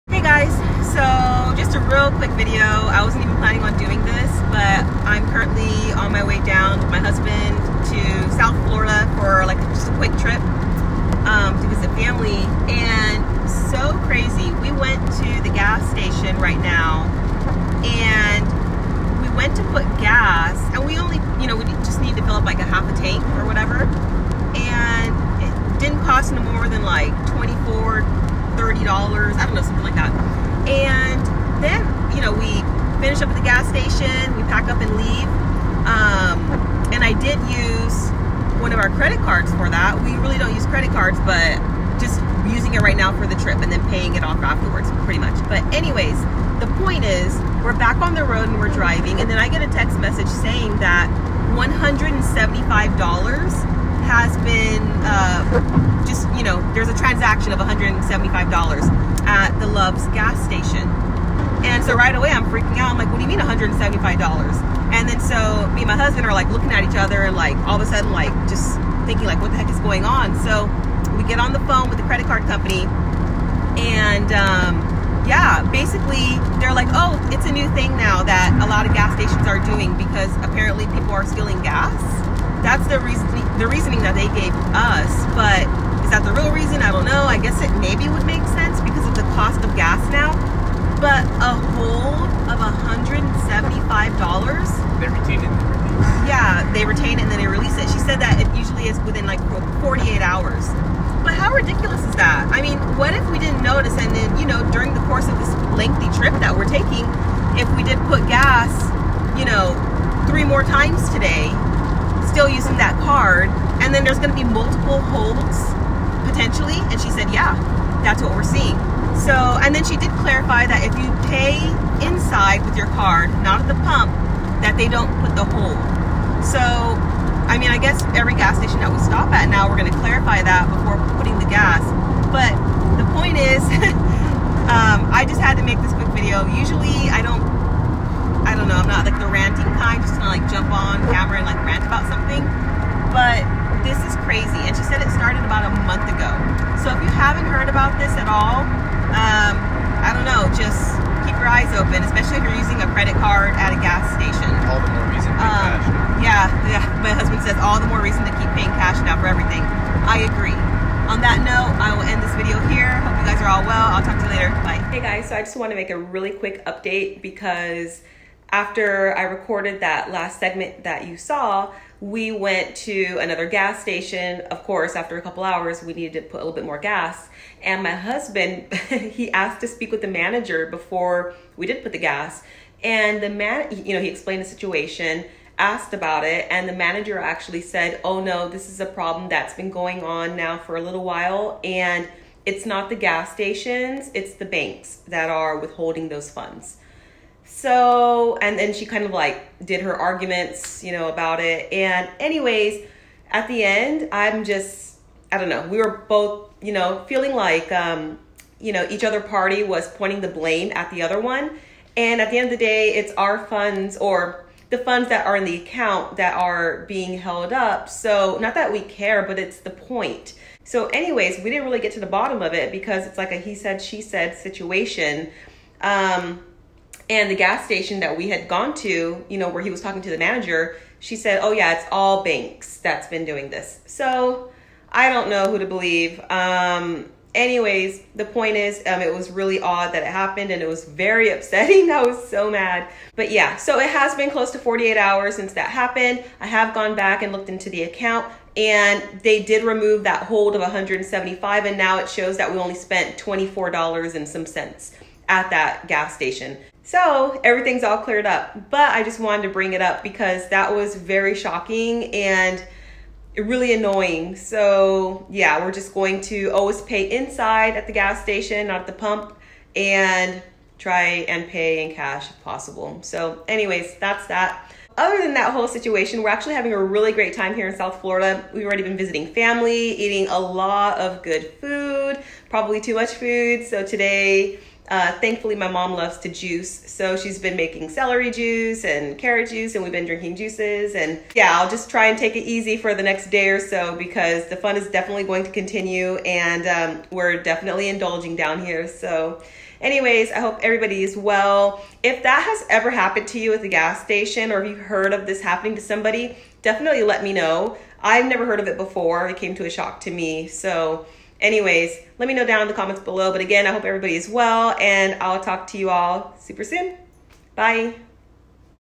Beware of authorization holds at gas stations | gas stations hold fees on cards 2022
Hey everyone - just wanted to give a quick heads up about what just happened to us at a major gas station. We couldn't believe it and wanted to share the information. This was the first time my husband or I ever experienced authorization holds, with the exception of hotel visits.

Let me know if you've experienced this before. Blessings!! ~ Barbara